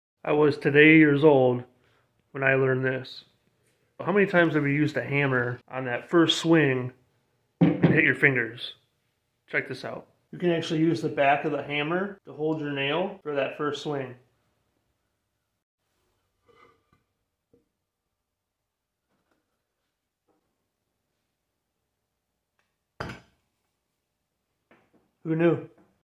Know This Hammer Trick? 🔨🤯
How many times have you hit your finger on that first swing. This hammer tip will prevent that from happening.